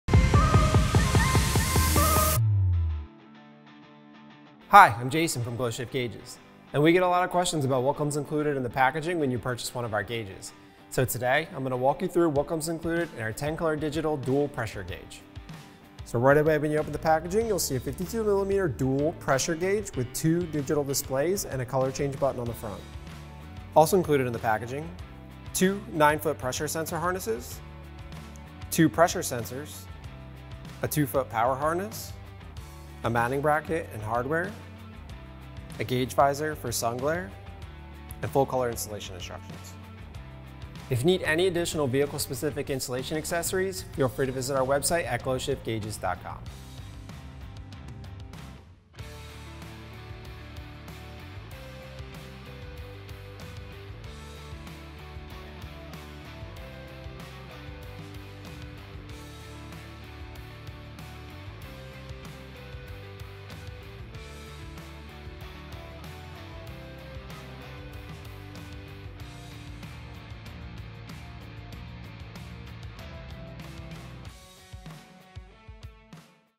Unboxing | GlowShift 10 Color Digital Series Dual Pressure Gauge for Cars and Trucks
Find out everything that’s included with GlowShift 10 Color Digital Series Dual Pressure Gauges that monitor two key pressure readings simultaneously.

DUAL PRESSURE GAUGE
In this GlowShift Unbox Series Video we’ll show you everything that comes with the purchase of our 10 Color Digital Series Dual Pressure Gauge Kit. This ultra-bright LED gauge with a dual digital display gives you the ability to monitor any combination of Boost, Oil Pressure and Fuel Pressure simultaneously from 0 to 100 PSI.

DIGITAL DUAL PRESSURE GAUGE KIT
When you open up the box, you’ll see the 2-1/16” (52mm) gauge. Also included are (2) 1/8-27 NPT Electronic Pressure Sensors; (2) 9’ Pressure Sensor Harnesses; a 2’ Power Harness; a Mounting Bracket and Hardware needed for the Dual Pressure Gauge install; a Gauge Visor that helps to block out any sun glare; and Installation Instructions.

10 COLOR DIGITAL SERIES
The 10 Color Digital Series features a flat black gauge dial and low profile tinted lens with bright LED digital numerical readouts. Each gauge has an ultra-thin design with a depth of 1-1/16” (27mm) and display that measures 3/8” x 5/8” (9.5mm x 15.92mm). Set the display to one of 10 available colors. At startup, Self-Calibration Mode ensures the gauge is performing optimally. For easier viewing at night, connect the orange wire to the headlight switch source and the gauge will dim 50%. GlowShift has you covered when you’re looking for the best air pressure gauge for your vehicle.